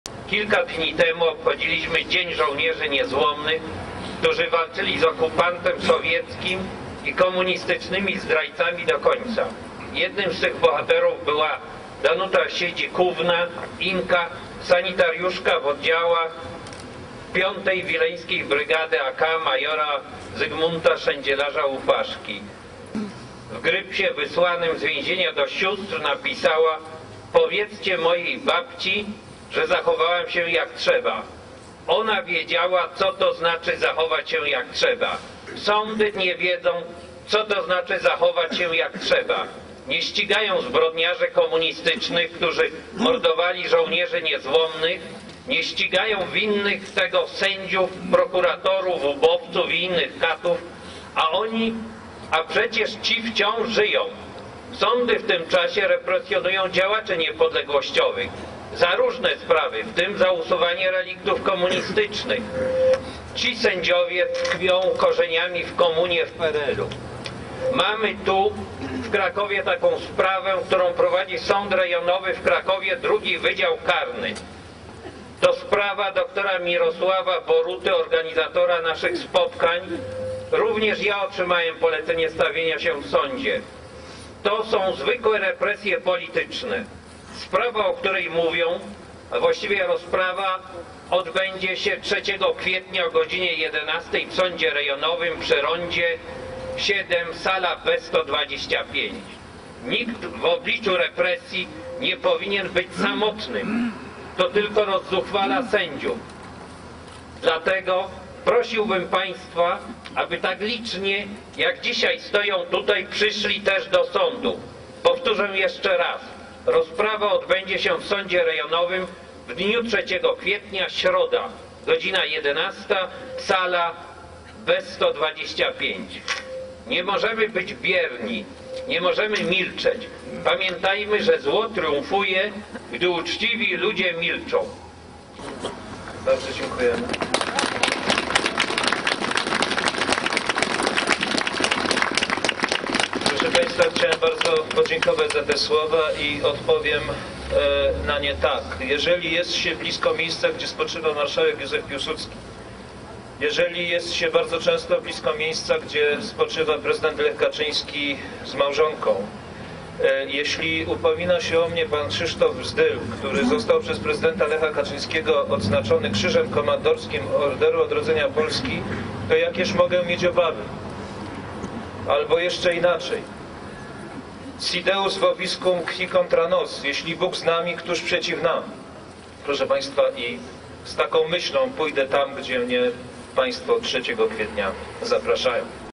Krakowska walka o Historie 2013 rok
Kraków walczyl o pamiec o zolnierzach wykletych zanim stalo to sie modne. Niejednokrotnie ludzie w tej walce byli osamotnieni jak Wieza Ratuszowa na Krakowskim Rynku ale z czasem zorganizowali sie w walce. Idea zjednoczyla ludzi.

SI DEUS VOBISCUM , QUIS CONTRA NOS.